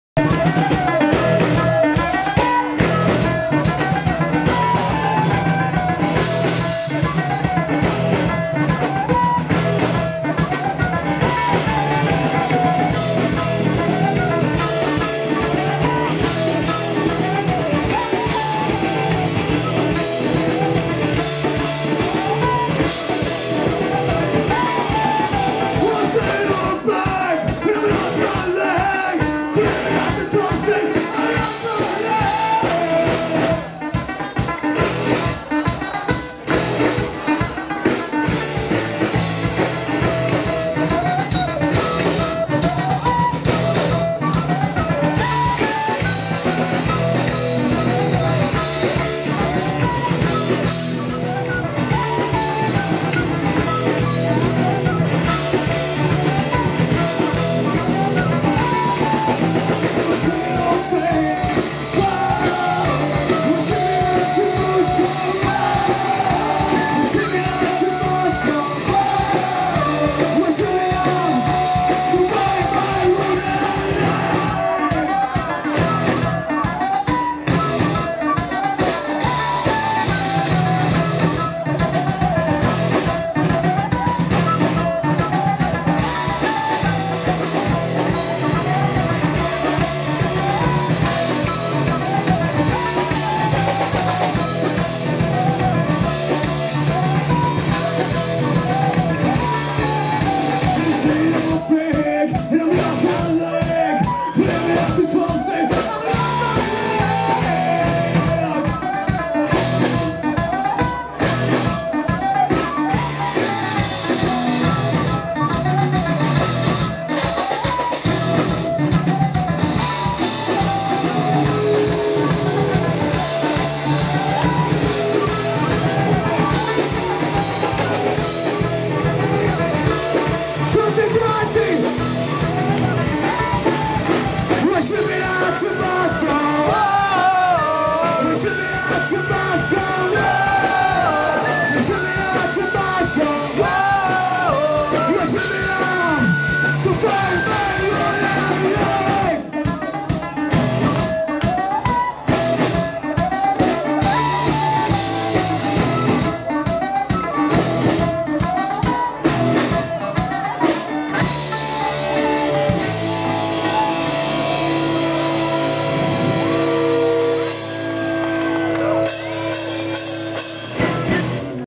Beltane 2011 - Uncle Bard & The Dirty Bastards 04
Uncle Bard & The Dirty Bastards alla festa di Beltane, a Masserano(Biella), 07/05/2011